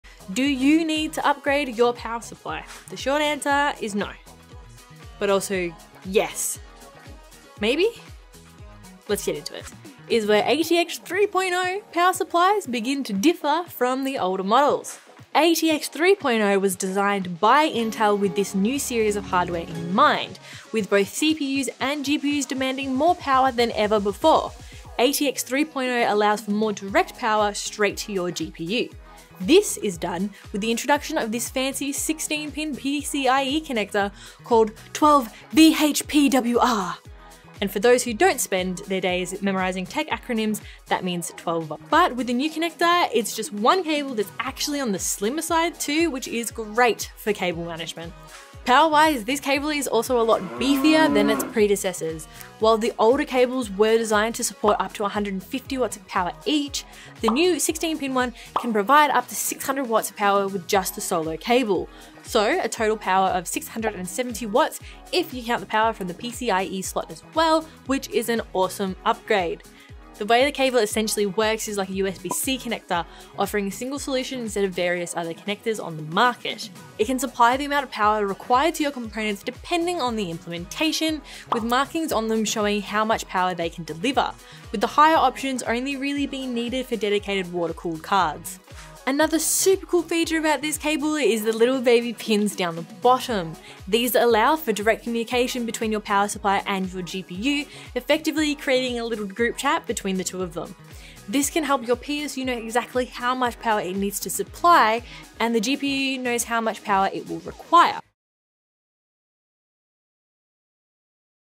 Do You Need An ATX 3 PSU Advert
It's almost been 20 years since the humble Power Supply has changed. With ATX 3.0 Power Supplies being released, you might be asking whether you need an ATX 3 PSU or not. Keely does a nice big deep dive for you, digging up all the info so you don't have to in this video. If you're thinking of upgrading to RTX 4000 series GPUs, you may want to give this a watch.